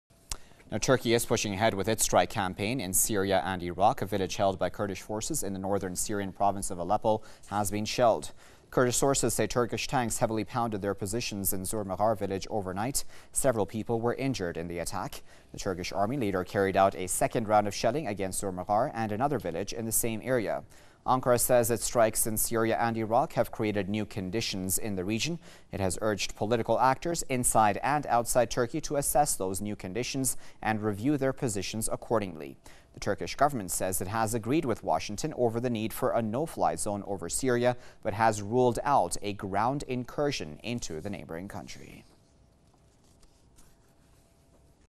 Turkey shells Kurdish village in Syria Report YouTube
Kurdish sources say Turkish tanks heavily pounded their positions in Zur Maghar village overnight. Several people were injured in the attack. The Turkish army later carried out a second round of shelling against Zur Maghar and another village in the same area. But a Turkish government official has denied targeting Syrian Kurds, saying the fighters’ positions remain outside the scope of Ankara’s current military action. Turkey says its strikes in Syria and Iraq have created new conditions in the region. The Turkish government says it has agreed with Washington over the need for a no-fly zone in Syria but has ruled out a ground incursion into the neighboring country.